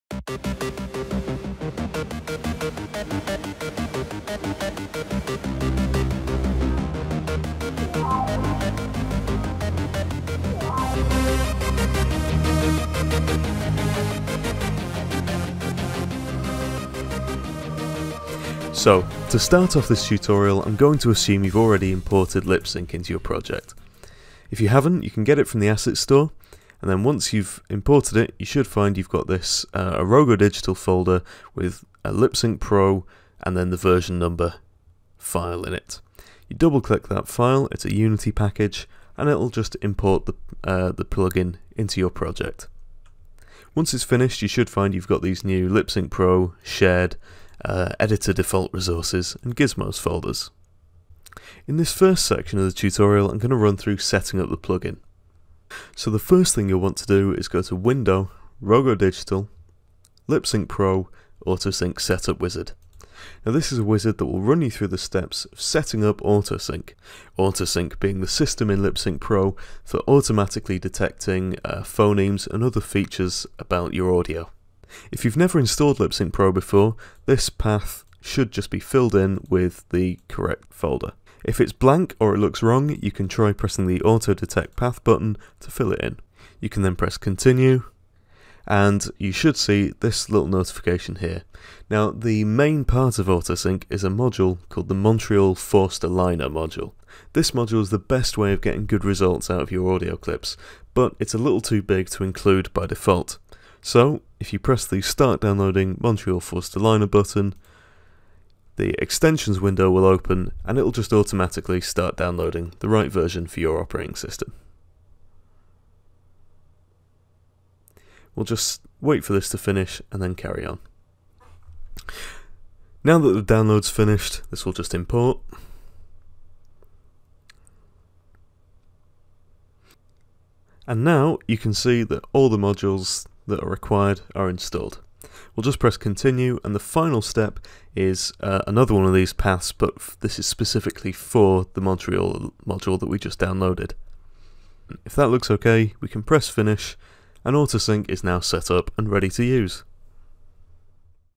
LipSync Pro Tutorials: Plugin Setup (2019)
This tutorial covers the basics of getting AutoSync set-up and ready to use, in LipSync Pro 1.5 and up.